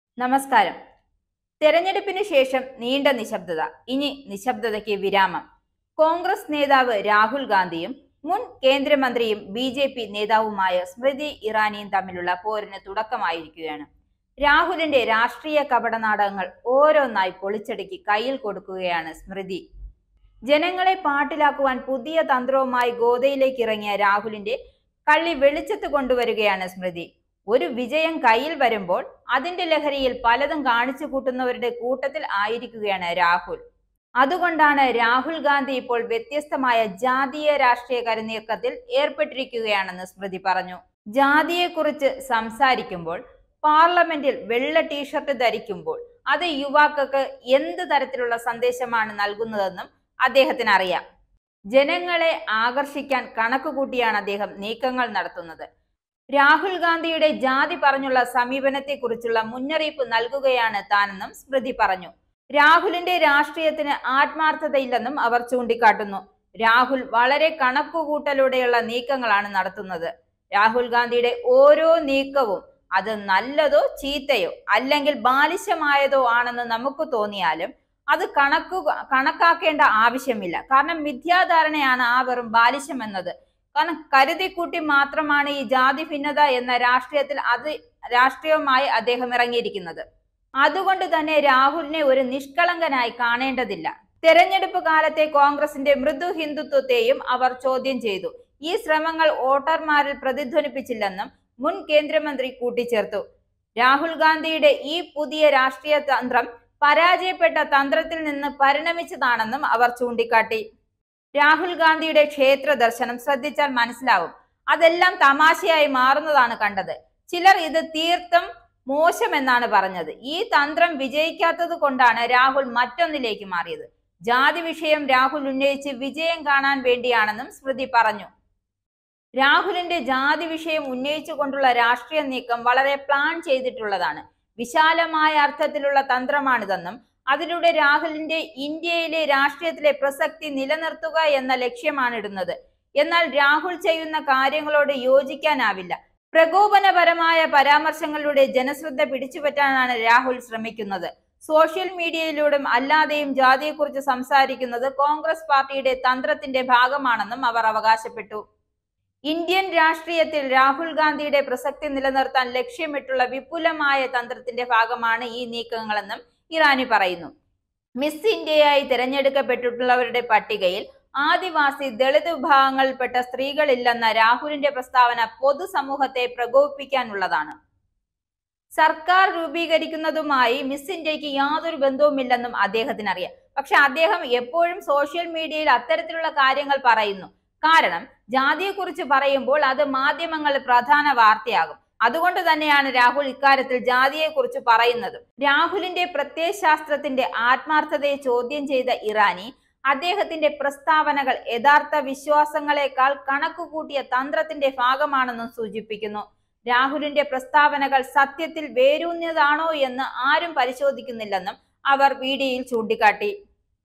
പപ്പുമോന്റെ ജാതിരാഷ്ട്രീയംപൊളിച്ചടുക്കി കയ്യിൽ കൊടുത്തു സ്മൃതി ഇറാനി | BJP
പപ്പുമോന്റെ ജാതിരാഷ്ട്രീയം
പൊളിച്ചടുക്കി കയ്യിൽ കൊടുത്തു സ്മൃതി ഇറാനി
പ്രകോപന പരാമര്‍ശങ്ങളിലൂടെ ജനശ്രദ്ധ പിടിച്ചുപറ്റാൻ തന്ത്രം
ജനങ്ങൾ മണ്ടൻമാരല്ലെന്ന് സ്മൃതി